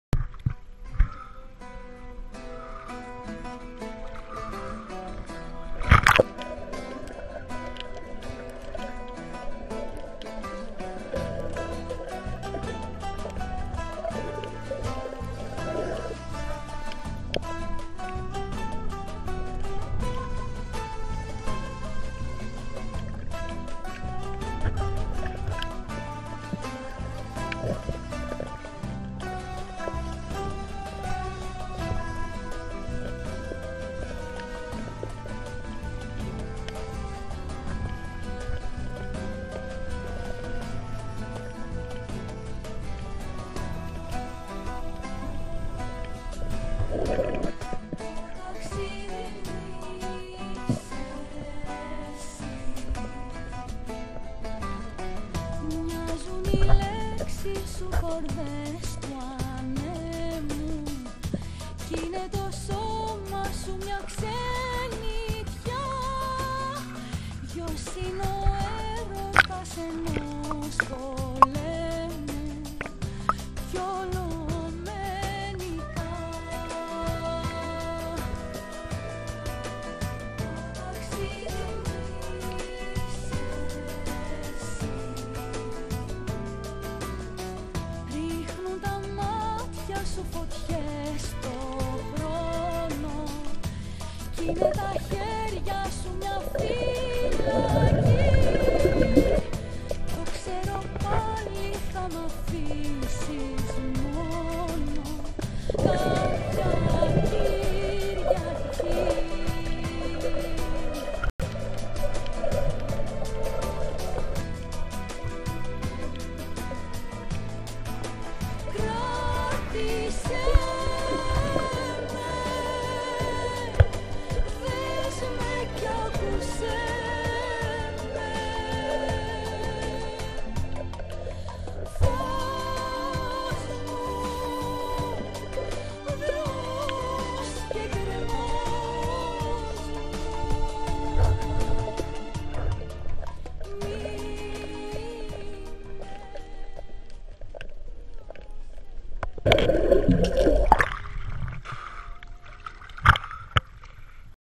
Korfu
Snorkeling i nurkowanie w Korfu, wrzesień 2014. Miejsca to głównie Porto Timoni/Afionas, ale także Kasspiopi, Agios Stefanos i Liapades.
Nakręcone z GoPro Hero3.

Snorkeling and freediving in Corfu, September 2014. Places being: mostly Porto Timoni/Afionas, but also Kassiopi, Agios Stefanos and Liapades. Shot with GoPro Hero3.